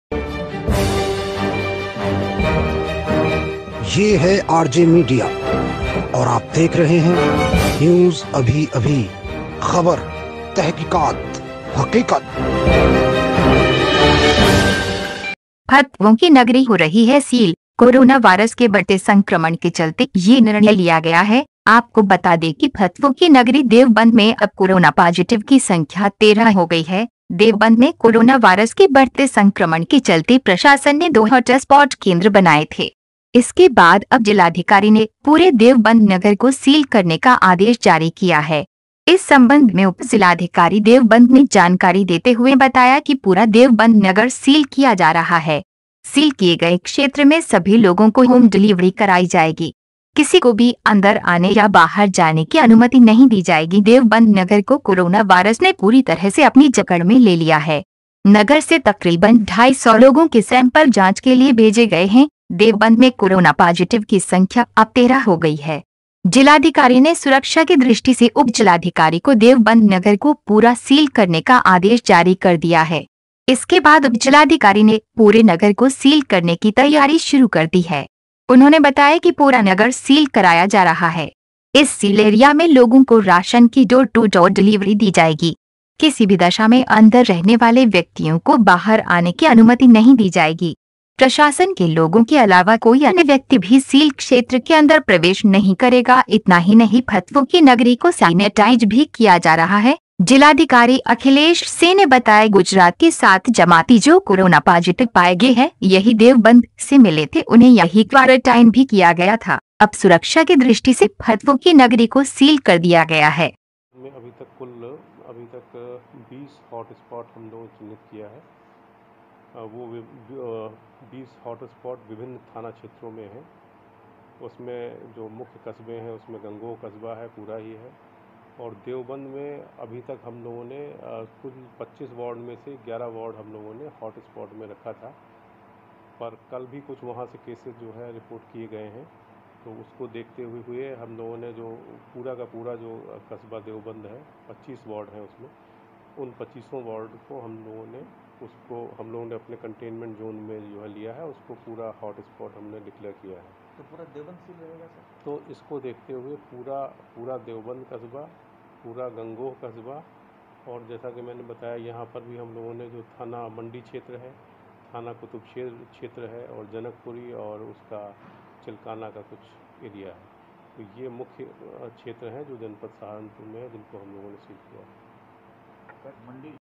देवबंद मे 13 हुये कोरोना पाजिटिव, फतवो की नगरी हुई सील
फतवों की नगरी हो रही है सील...कोरोना वायरस के बढते संक्रमण के चलते ये निर्णय लिया गया है...आपको बता दे कि फतवो की नगरी देवबंद मे अब कोरोना पाजिटिव की संख्या 13 हो गयी है..देवबंद में कोरोना वायरस के बढ़ते संक्रमण के चलते प्रशासन ने दो हॉटस्पॉट केंद्र बनाए थे. इसके बाद अब जिलाधिकारी ने पूरे देवबंद नगर को सील करने का आदेश जारी किया है. इस संबंध में उप जिलाधिकारी देवबंद ने जानकारी देते हुए बताया कि पूरा देवबंद नगर सील किया जा रहा है. सील किए गए क्षेत्र में सभी लोगों को होम डिलीवरी कराई जाएगी. किसी को भी अंदर आने या बाहर जाने की अनुमति नहीं दी जाएगी.देवबंद नगर को कोरोना वायरस ने पूरी तरह से अपनी जकड़ में ले लिया है. नगर से तकरीबन ढाई सौ लोगों के सैंपल जांच के लिए भेजे गए हैं ...देवबंद मे कोरोना पाजिटिव की संख्या अब13 हो गयी है।

जिलाधिकारी ने सुरक्षा की दृष्टि से उपजिलाधिकारी को देवबंद नगर को पूरा सील करने का आदेश जारी कर दिया है. इसके बाद उपजिलाधिकारी ने पूरे नगर को सील करने की तैयारी शुरू कर दी है. उन्होंने बताया कि पूरा नगर सील कराया जा रहा है. इस सील एरिया में लोगों को राशन की डोर टू डोर डिलीवरी दी जाएगी. किसी भी दशा में अंदर रहने वाले व्यक्तियों को बाहर आने की अनुमति नहीं दी जाएगी. प्रशासन के लोगों के अलावा कोई अन्य व्यक्ति भी सील क्षेत्र के अंदर प्रवेश नहीं करेगा.इतना ही नही फतवो की नगरी को सेनेटाइज भी किया जा रहा है..जिलाधिकारी अखिलेश सिंह ने बताया गुजरात के 7जमाती जो कोरोना पाजिटिव पाये गये है यही देवबंद से मिले थे उन्हे यही क्वारेटाइन भी किया गया था...अब सुरक्षा की दृष्टि से फतवो की नगरी को सील कर दिया गया है